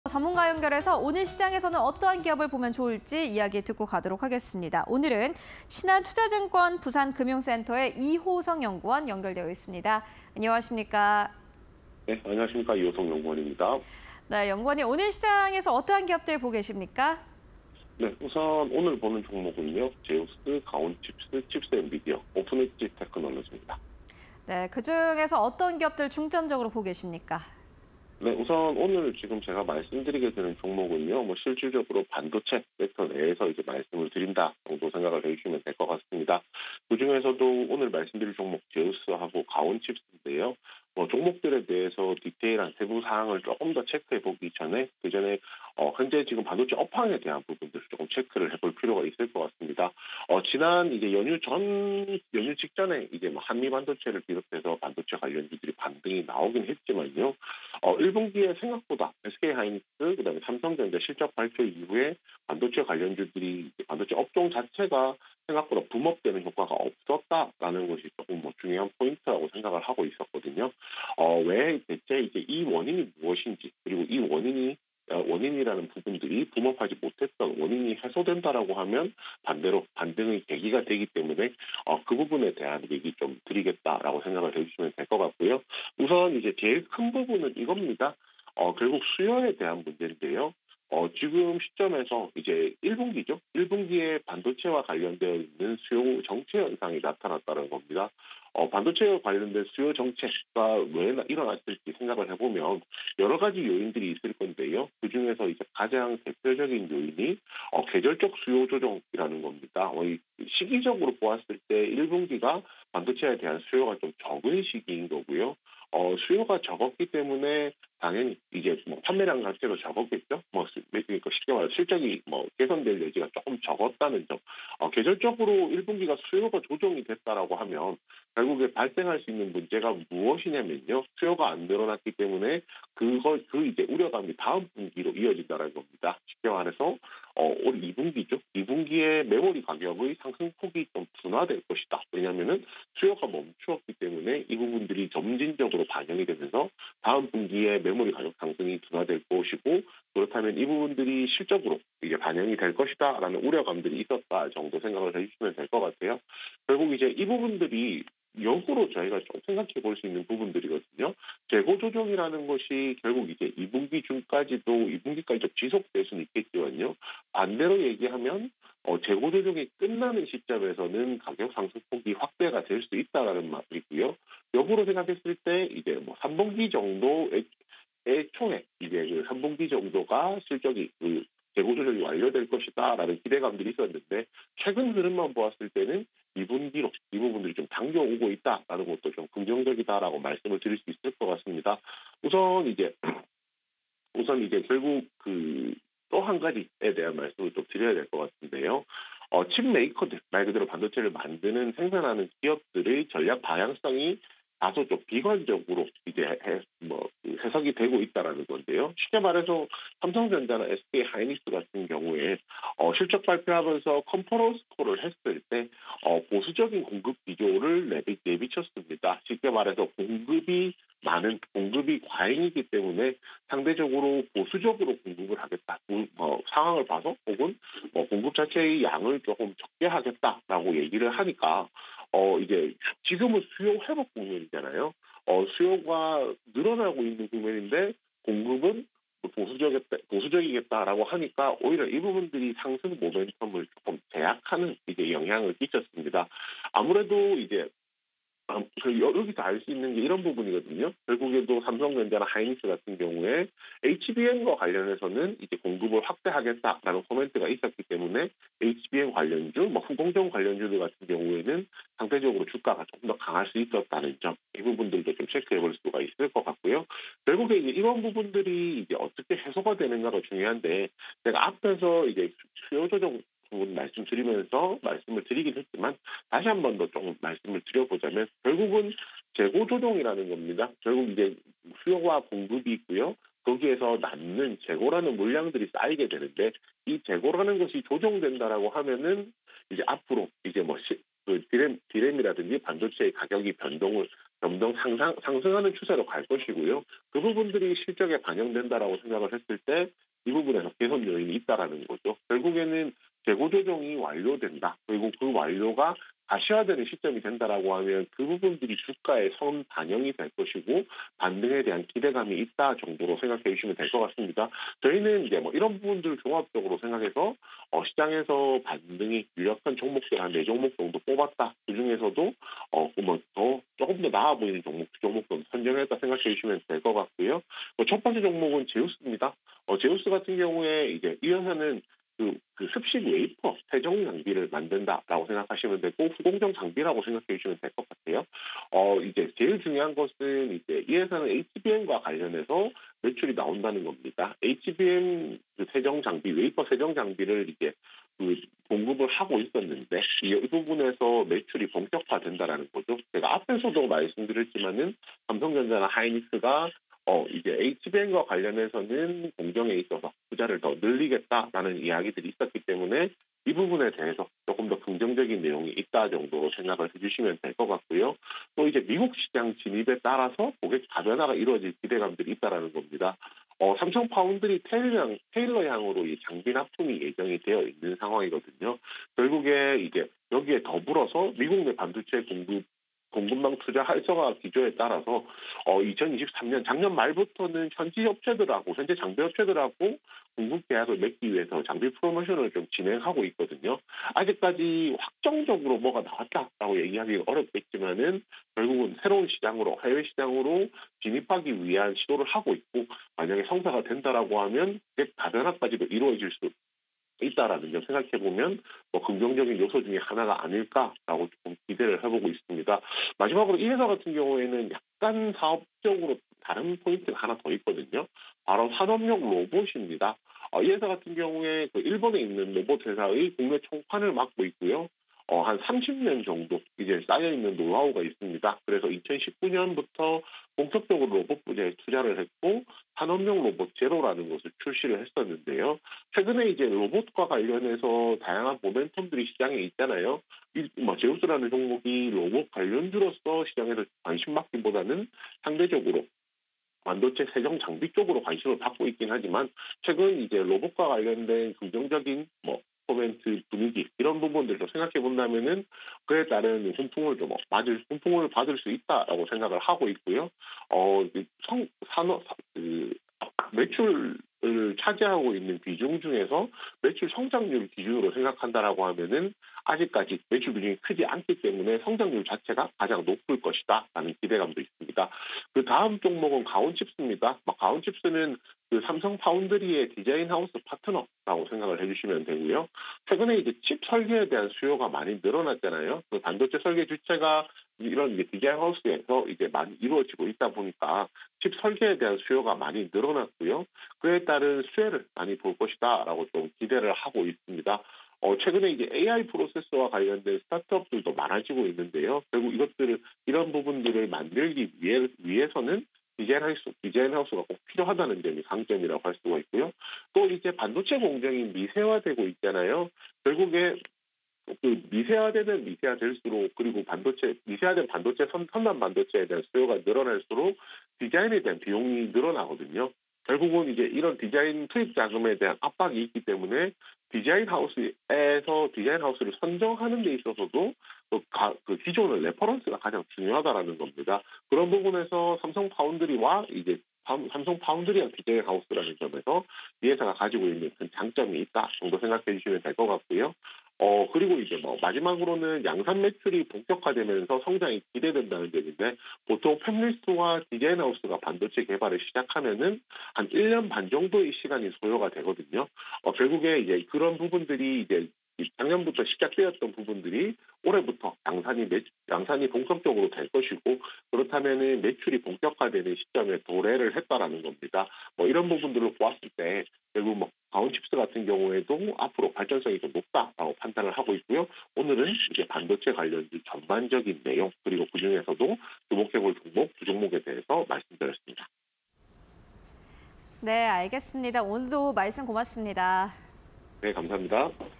제우스, 가온칩스, 칩스앤미디어, 오픈엣지테크놀로지_이호석의 주력 종목 (20240213)
1. 제우스(079370)
2. 가온칩스(399720)
3. 칩스앤미디어(094360)
4. 오픈엣지테크놀로지(394280)

제우스(079370)
- HBM·웨이퍼 등 세정 장비 매출 본격화
- 美 시장 진출 따른 고객 다변화 기대
- 산업용 로봇 사업 본격 확대

가온칩스(399720)
- 시스템반도체 전문 디자인 솔루션 업체
- 턴키 방식 사업... 완제품 형태로 공급
- 경쟁사 대비 높은 기술적 진입 장벽 확보
- 칩 설계 수요 증가 수혜 예상
- 반도체 공정 선단화 수혜
- 양산 매출 본격화 따른 성장 기대
- 美 실리콘밸리 법인 설립